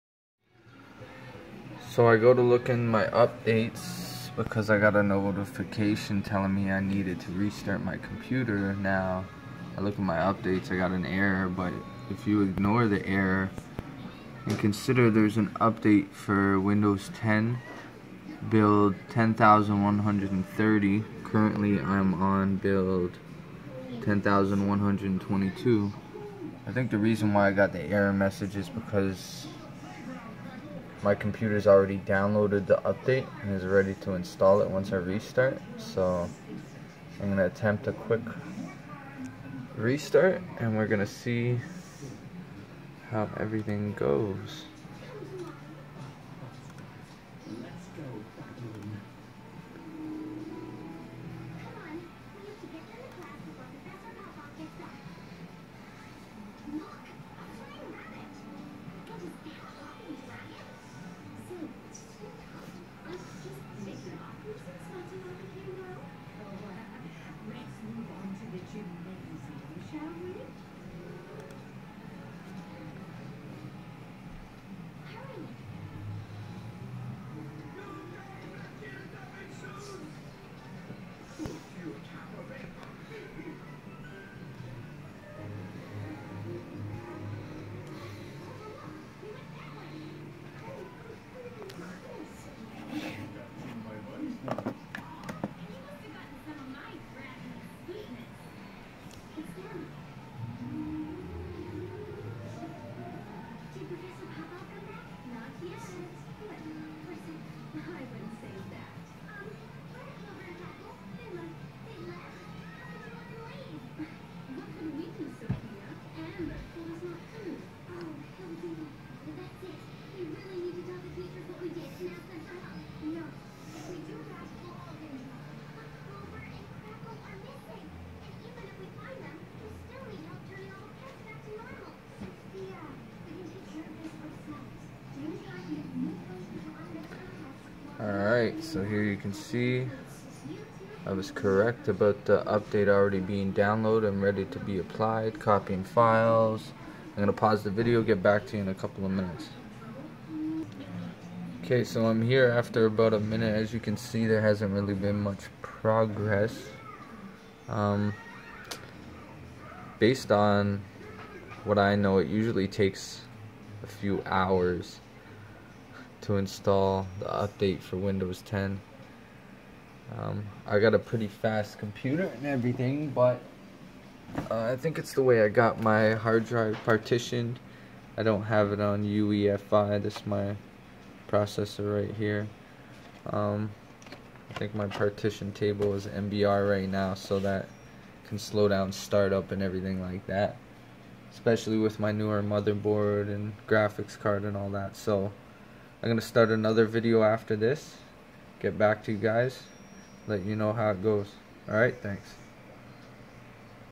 WINDOWS 10 BUILD 10130 UPDATE AND OVERVIEW Part 1
Hello this is a 3 part video showing the update process and overview of Windows 10 Build 10130 which is the latest update released by Microsoft.

2ND PART OF THE VIDEO WILL BE UP AS SOON AS POSSIBLE. HAD A HUGE ISSUE LEFT WITH NO INTERNET CONNECTION/LIMITED. Resetting pc settings to fix hopefully all goes well. Virtualbox being installed before the update most likely cause the Internet connection issue in Windows 10.

UPDATE VIDEOS 2 AND 3 HAVE BEEN UPGRADED SORRY FOR THE DELAY.

My setup is as fallows.

Newer Asus Motherboard (Will get back on which version shortly)

AMD R9 Graphics Card

Cooler Master Desktop Case

MBR Partition table which I would like to switch to UEFI once I have all my files backed up. UEFI should be faster all around.

Click or watch an ad two to show support.

Thanks and have a wonderful day.